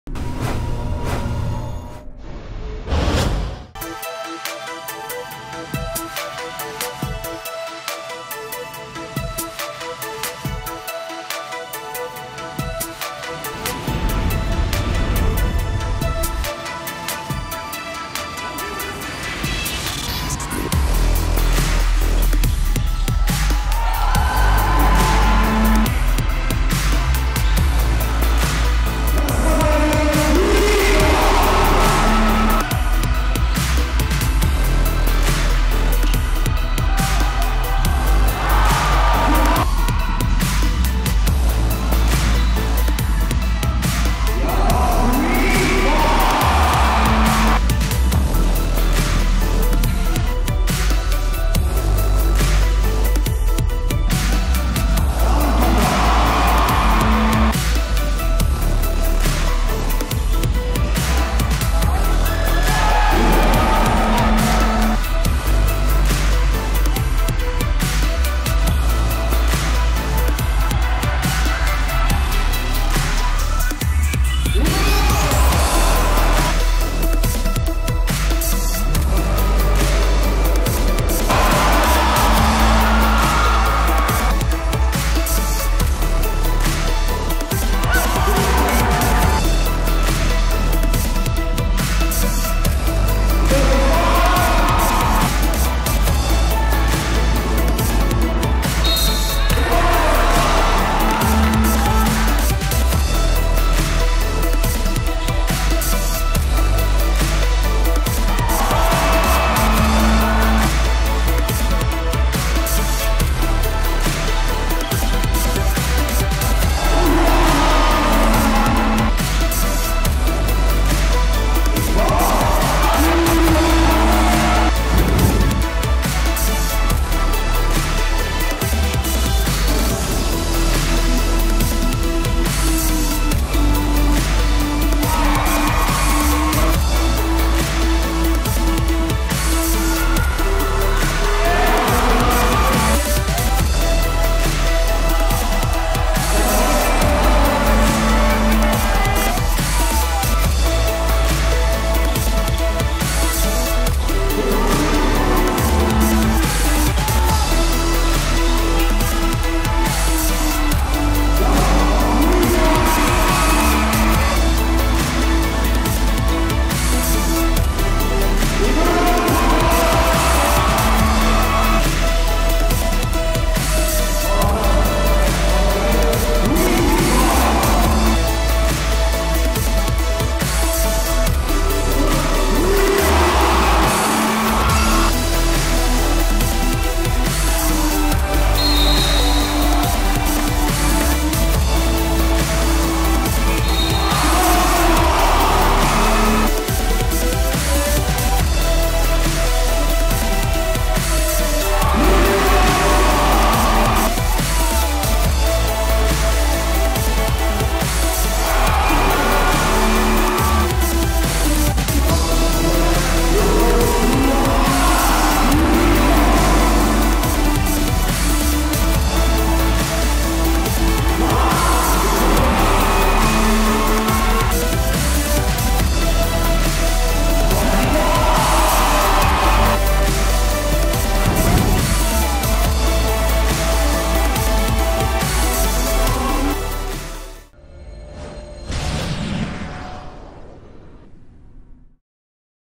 NBL Indonesia Series IV Game #5 JNE BSC Bandung Utama Vs Satya Wacana Metro LBC Bandung
Game Highlights
Game #5 NBL Indonesia season 2013/14
Round 4 Bandung 13th March 2014
Arena: C-Tra Arena Bandung
Final Score: JNE BSC BANDUNG UTAMA (92)  v (69) SATYA WACANA METRO LBC BANDUNG